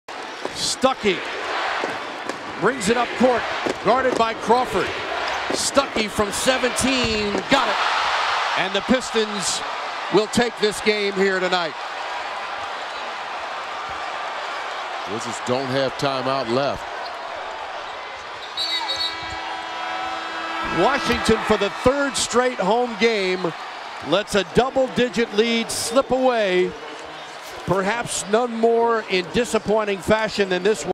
Stuckey's Game Winner!
Check out Pistons guard Rodney Stuckey as he shows off his composure with this late game drive and jumper to give the Pistons the win!